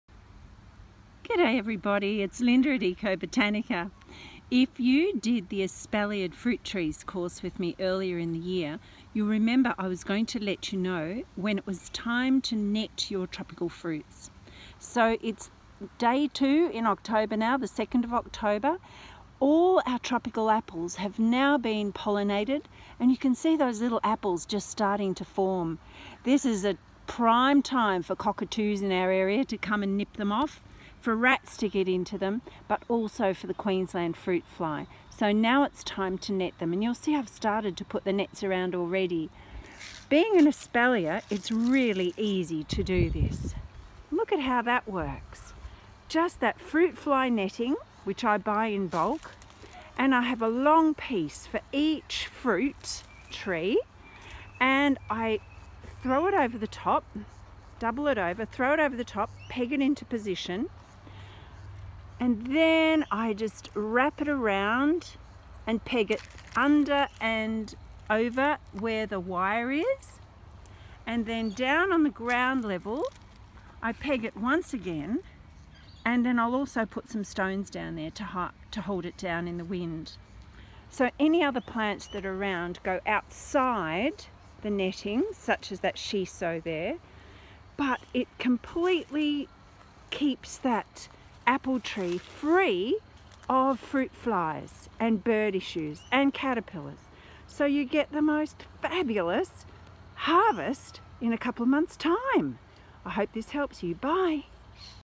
Taking Care of Espalier Fruit Trees.
How to protect your espaliered apples, & other fruits from cockatoos, possums & fruit flies.